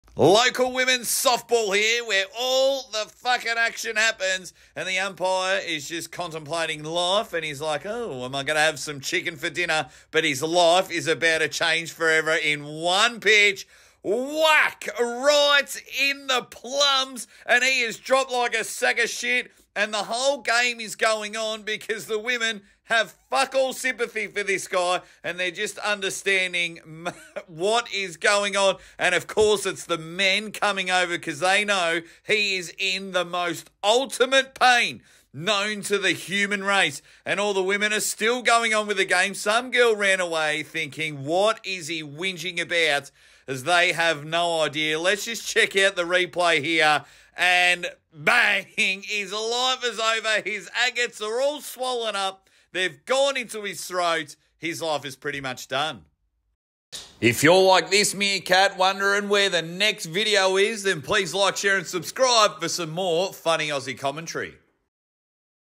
Umpire gets hit in the balls | NO ONE CARES
Local softball umpire gets hit in the balls with no one caring 🤣🤣 also with a bit of fun Aussie commentary